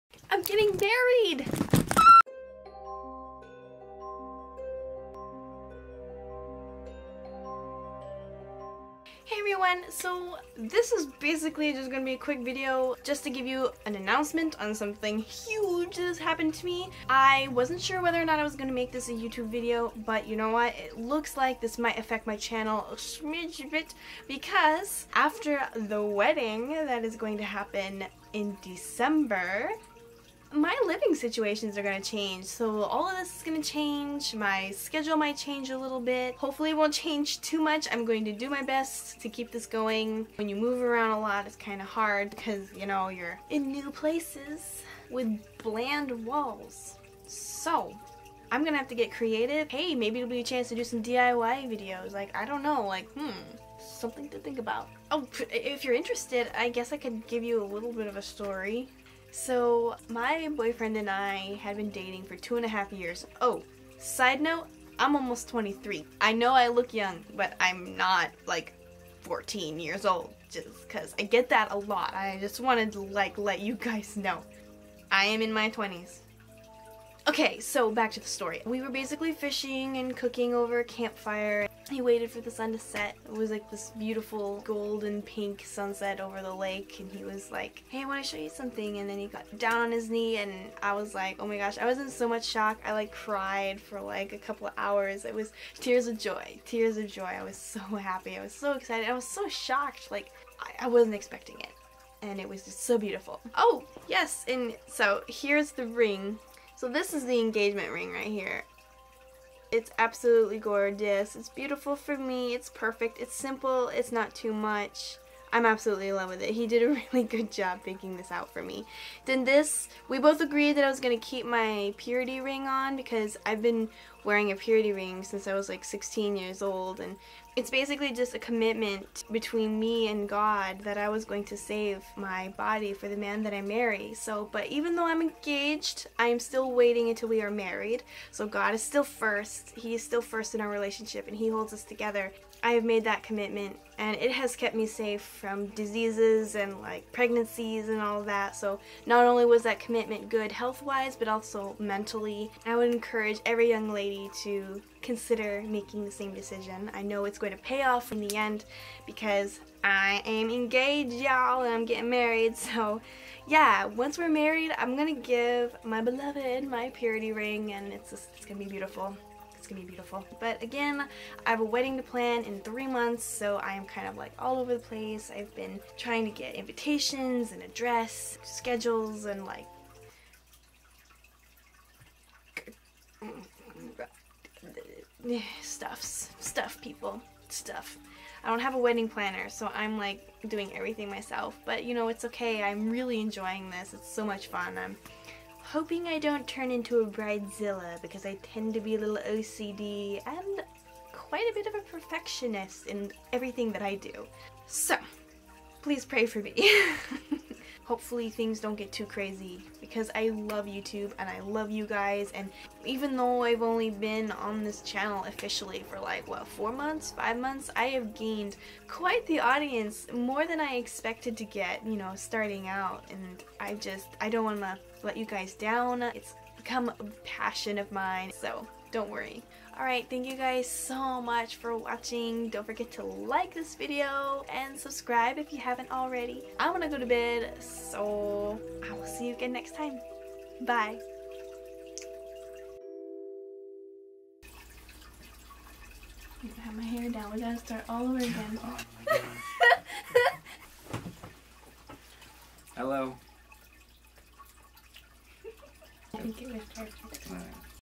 Getting Married Announcement! + Story Time
It's true! I'm getting married!!! Watch all the way to the end to meet my handsome fiance ;)